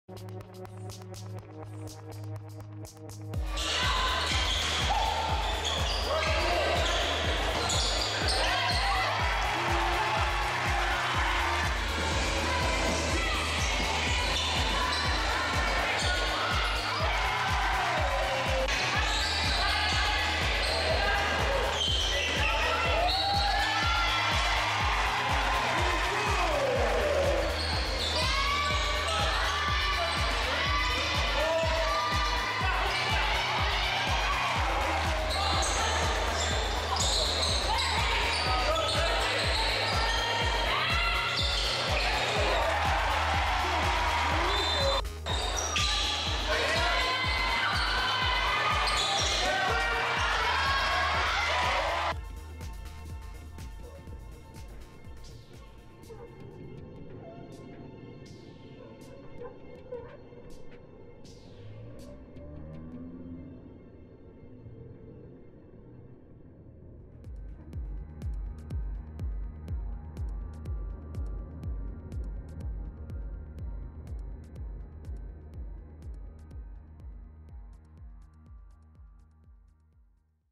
Highlights Sydney Vs Perth Round 4
In Round 4 Sydney lost in a hard fought battle 62-78 to ladder leaders Perth Lynx.
The final score did not reflect how much the Flames were in this game, until the final minutes. Whilst recovering from injuries & still missing a critical piece to our squad in Tahlia Tupaea, this game highlighted the pesky fight the Flames have maintained throughout despite major set-backs to the start of our season.